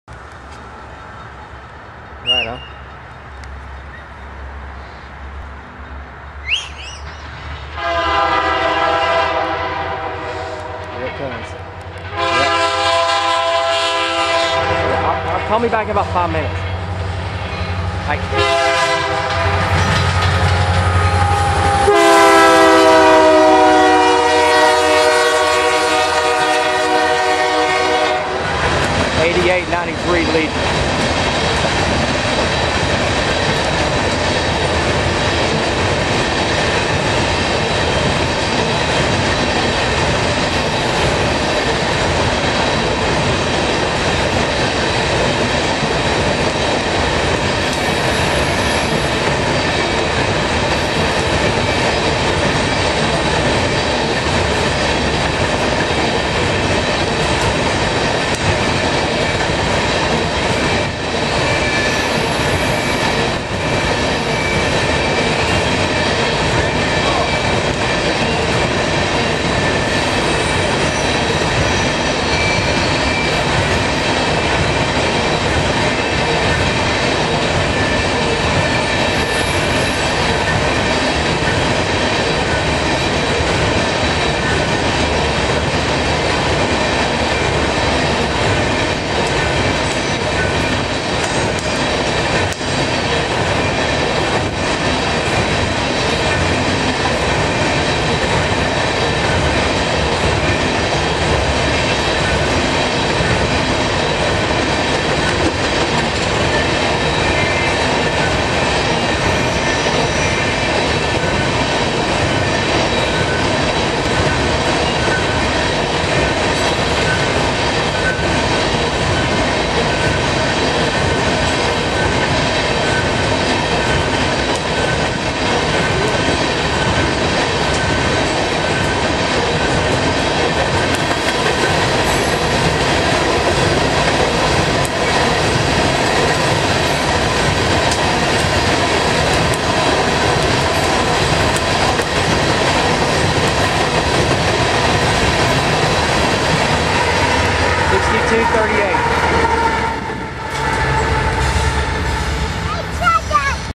Norfolk Southern 739 NB BNSF Scherer Coal in Austell,Ga 12-02-2012© (16x9)
7:37PM in Austell,Ga is NS 739 Northbound BNSF Scherer coal train with friendly crew!

BNSF 8898 SD70MAC
BNSF 5839 ES44AC
BNSF 6238 ES44AC (DPU)

Recorded by Gene and Tommy Hale on December 2, 2012